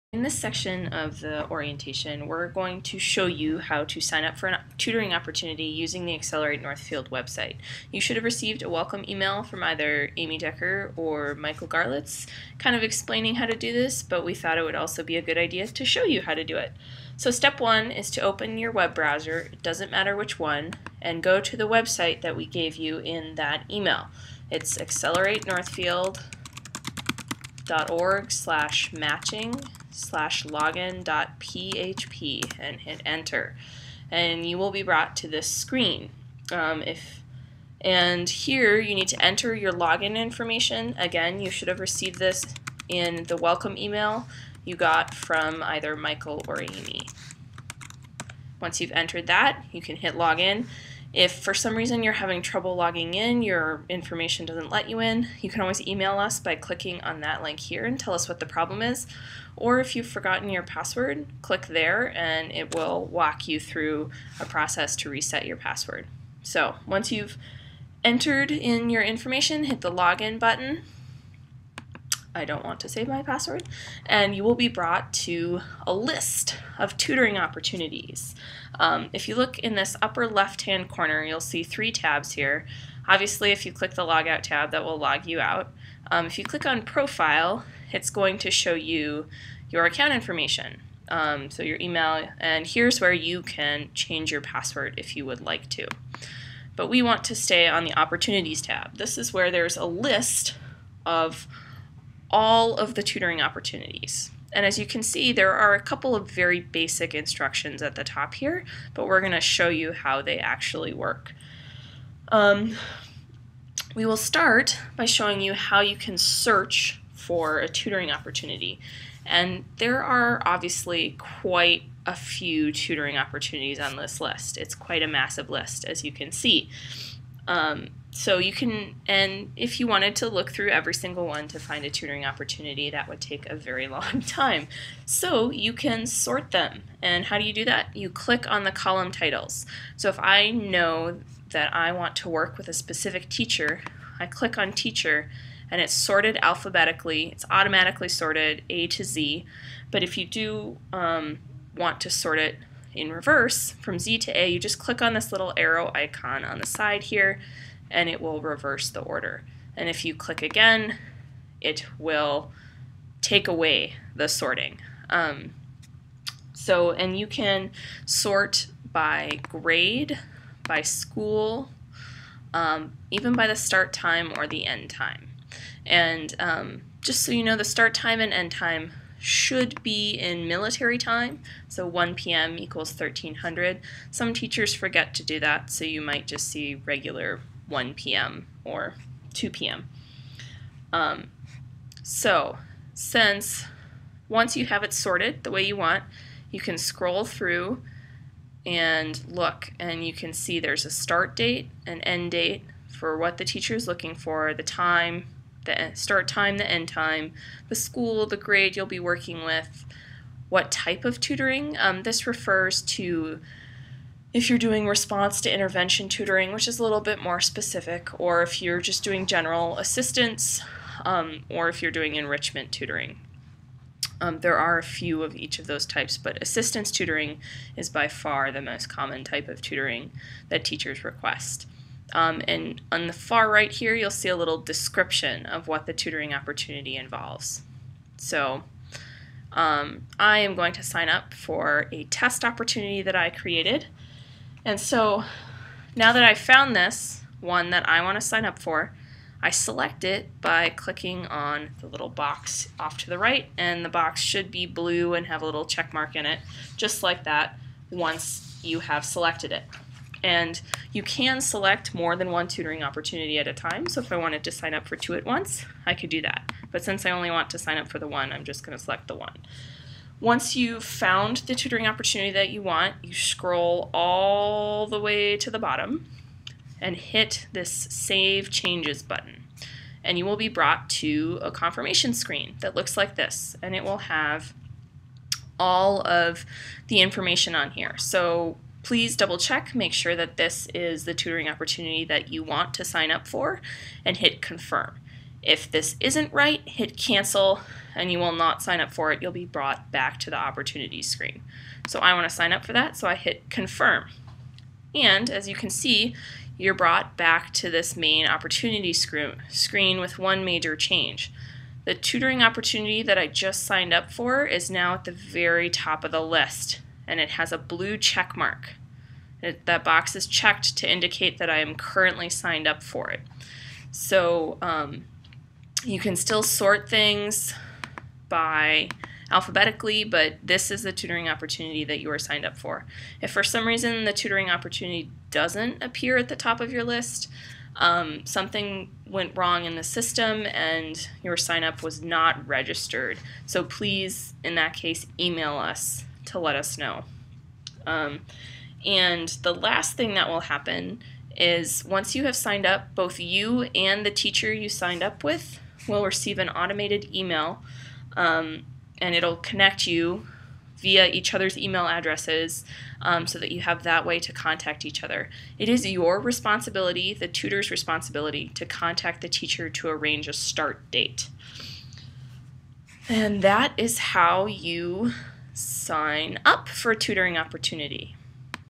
Tutoring Opportunity Sign Up
How to use our online system to sign up for a tutoring opportunity.